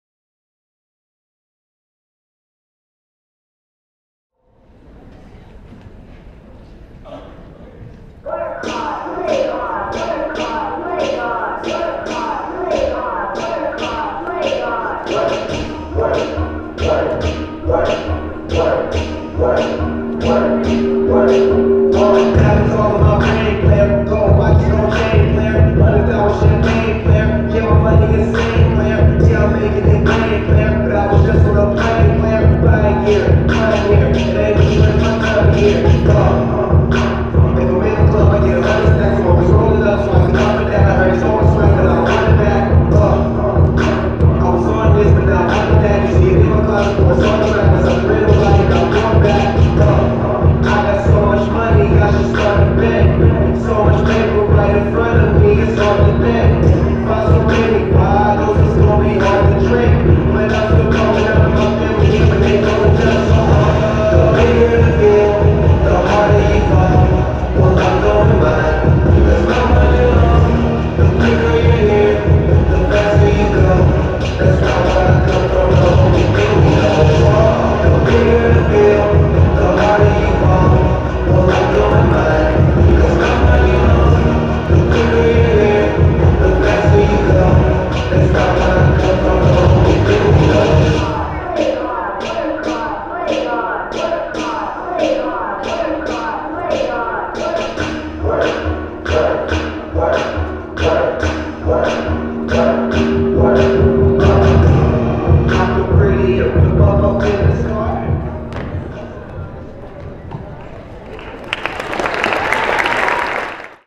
Who's the Bully? - Barton Dance
Barton Dance as Art and Culture final project performances. Barton Uppers Exhibition, January 21, 2014.